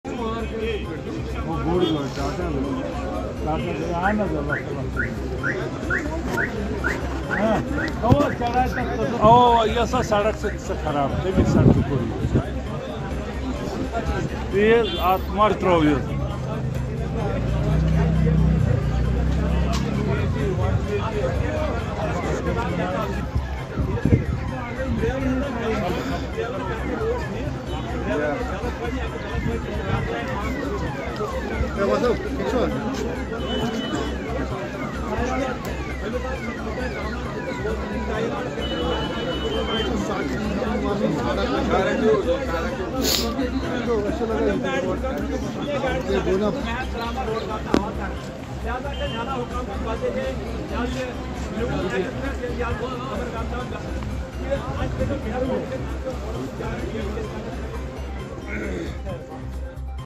Six injured in a bus accident at Bijbehara Anantnag
Six injured in a bus accident at Bijbehara Anantnag, Injured were treated at Sub-District Hospital